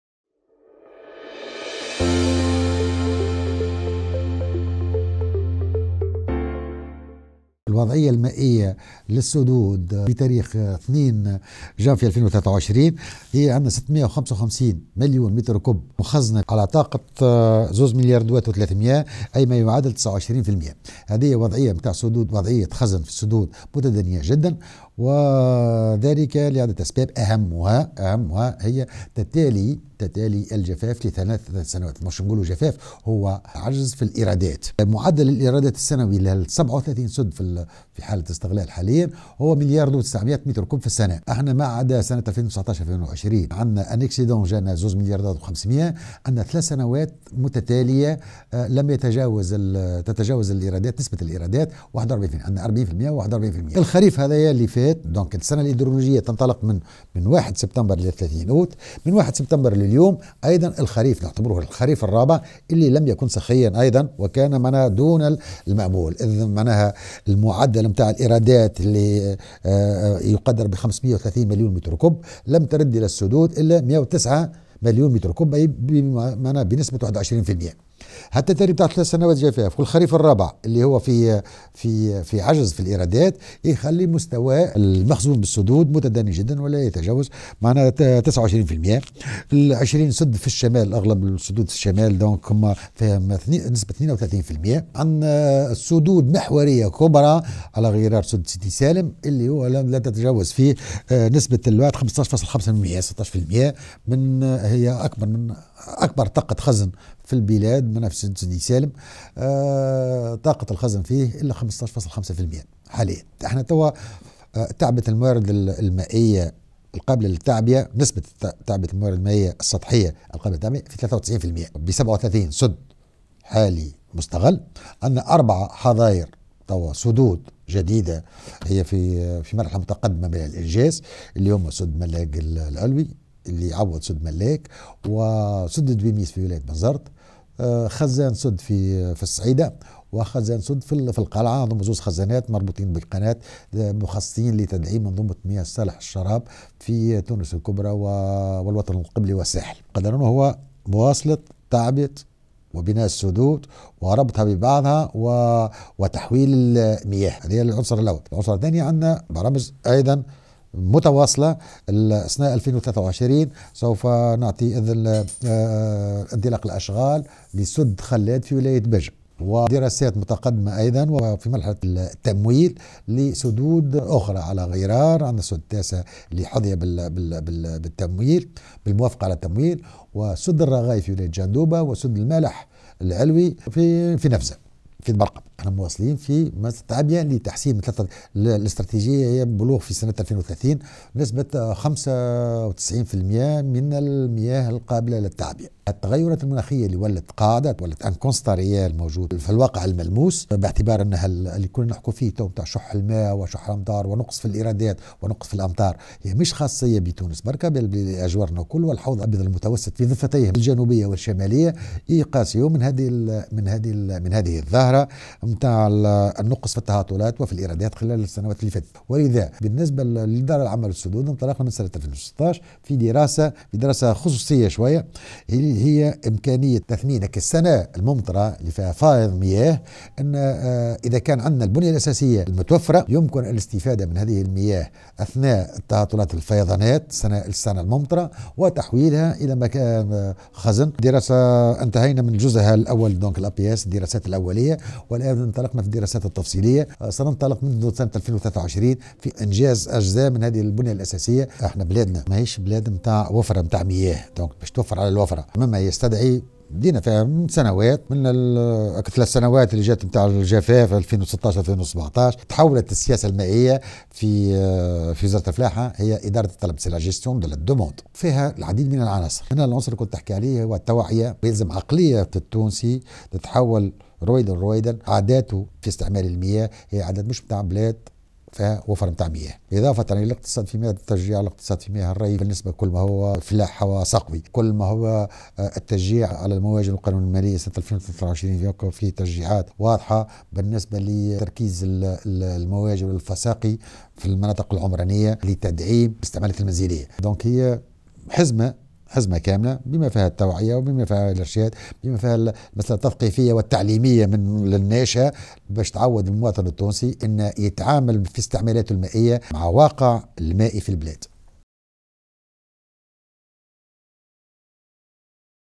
فائز مسلم: استراتيجية وزارة الفلاحة لسنة 2030 بلوغ نسبة 95 % من المياه القابلة للتعبئة
مدير عام الأشغال المائية الكبرى: استراتيجية وزارة الفلاحة لسنة 2030 بلوغ نسبة 95 % من المياه القابلة للتعبئة